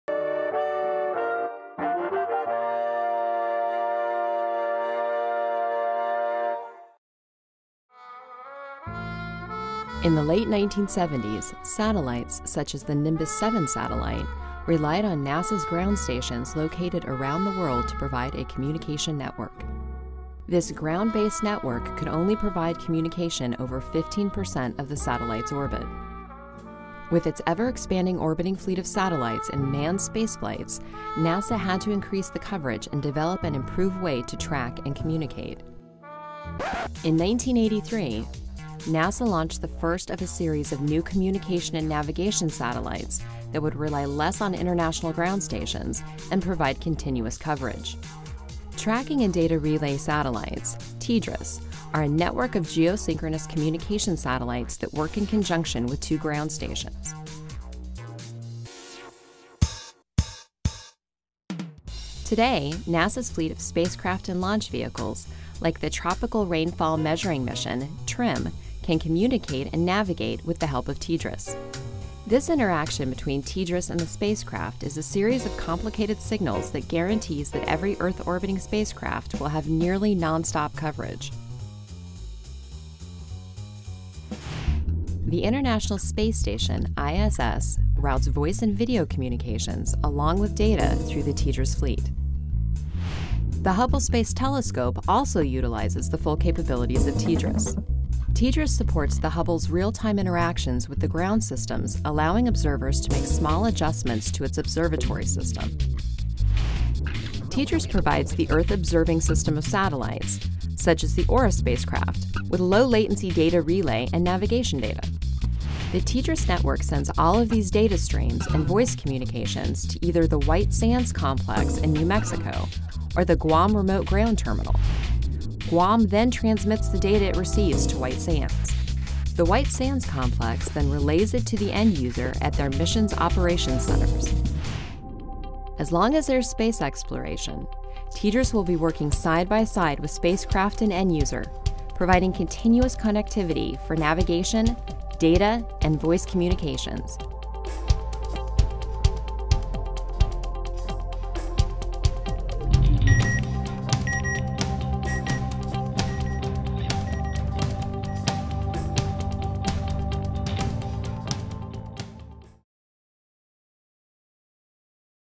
NASA | TDRS: The Network That Enables Exploration
NASA is preparing to launch the second in a series of three, third generation advanced Tracking and Data Relay Satellites, known as TDRS. This latest addition to the fleet of eight, TDRS-L will augment a space communications network that provides the critical path for high data-rate communication to the International Space Station, Hubble Space Telescope, human occupied spacecraft and a host other spacecraft.